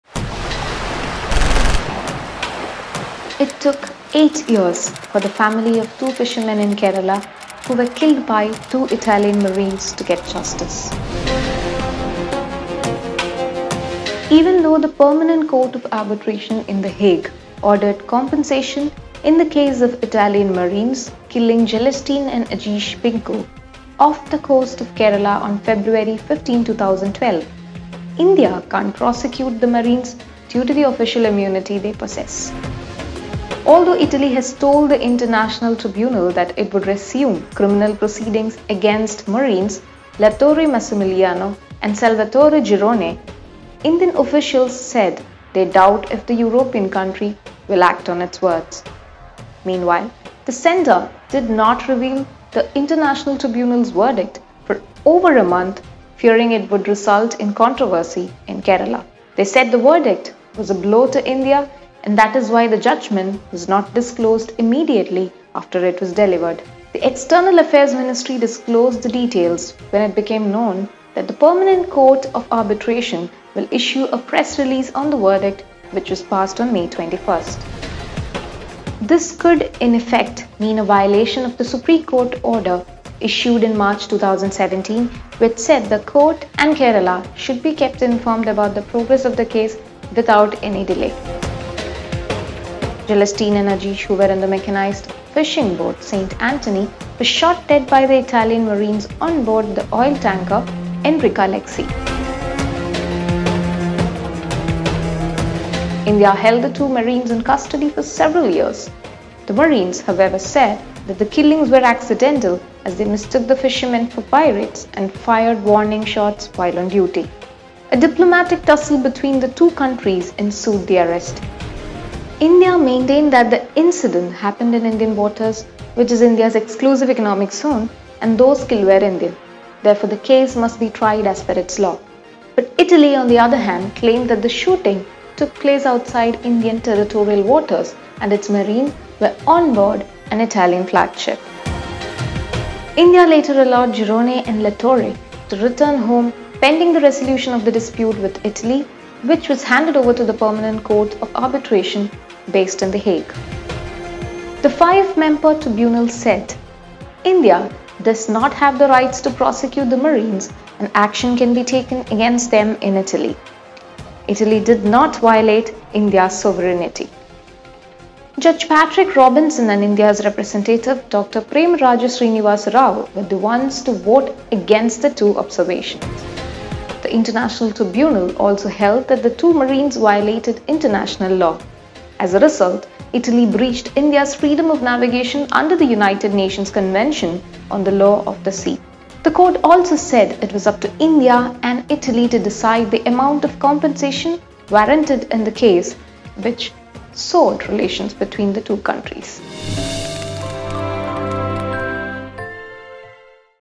What happened at the Hague in Enrica Lexie case
Though the Permanent Court of Arbitration in the Hague ordered compensation for the killing of Jelestine and Ajeesh Pinku by Italian marines off the coast of Kerala, India can't prosecute the marines due to official immunity enjoyed by them.